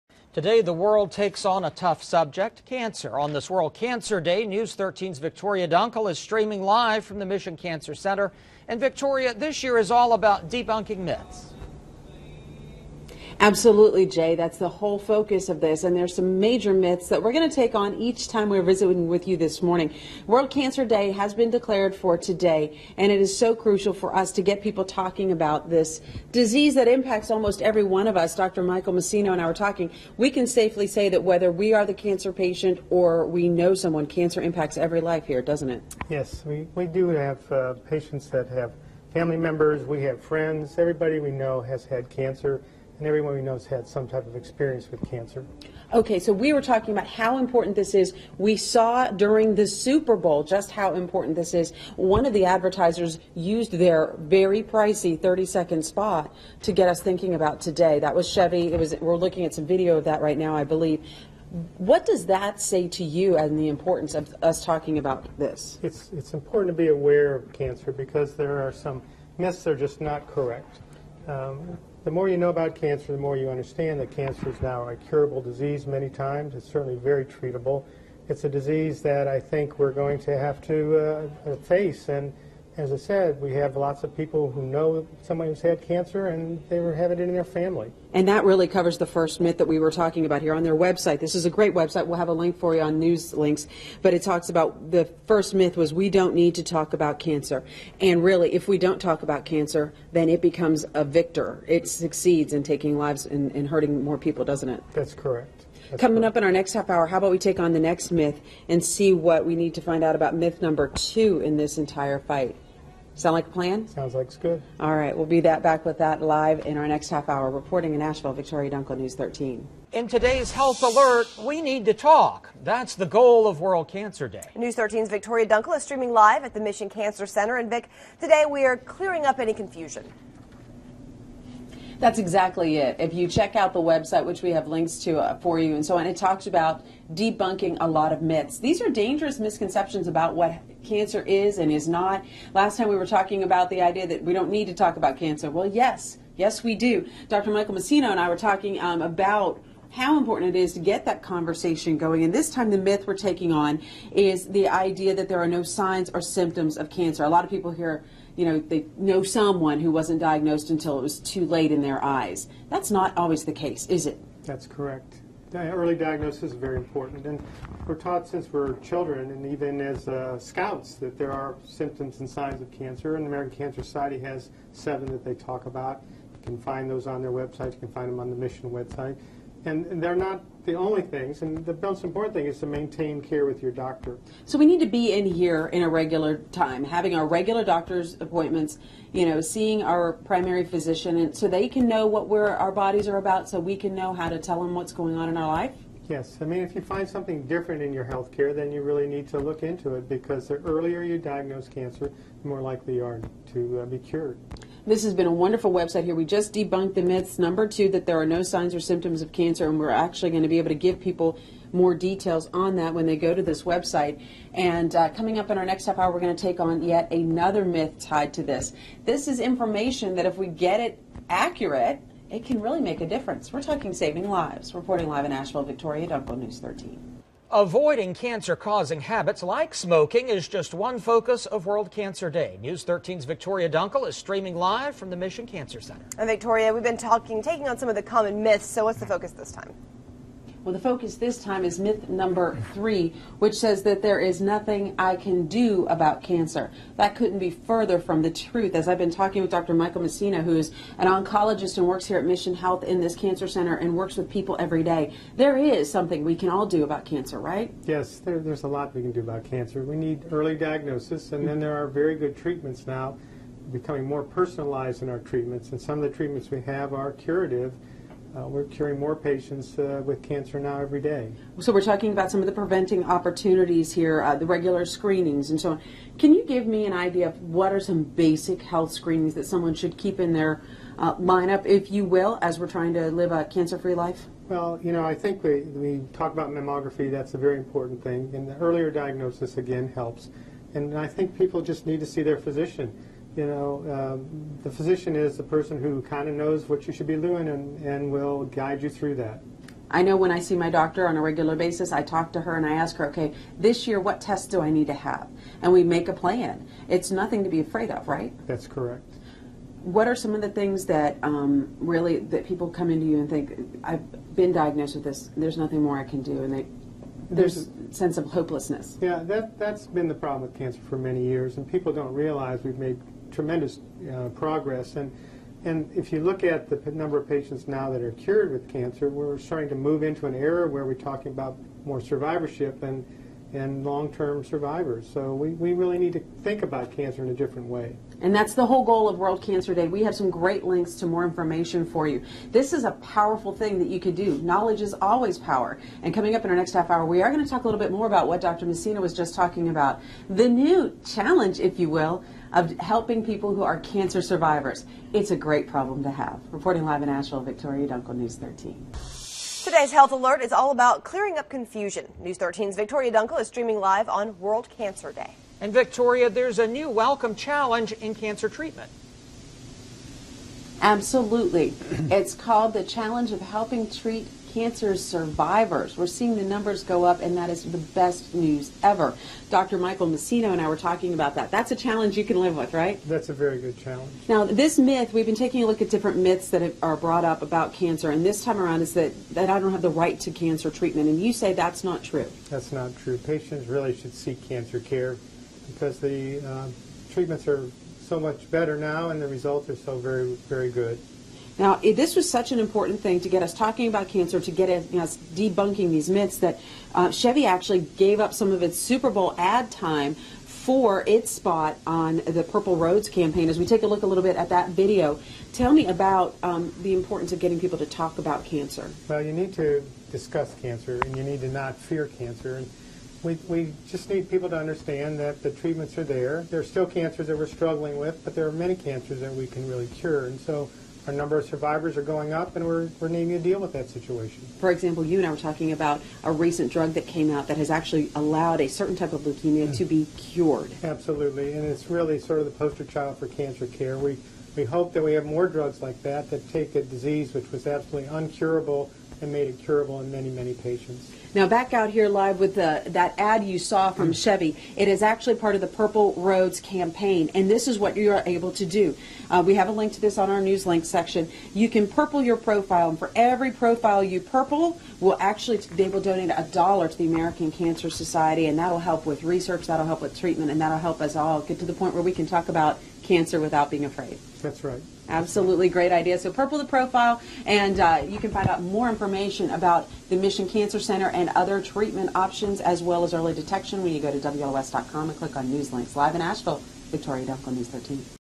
World Cancer Day
We need to talk, about cancer. Today is World Cancer Day. It is a day devoted to clearing up confusion about the diseases that impact virtually all of us directly or indirectly.

Today on News 13 This Morning, News 13's Victoria Dunkle took the most common myths to Mission Health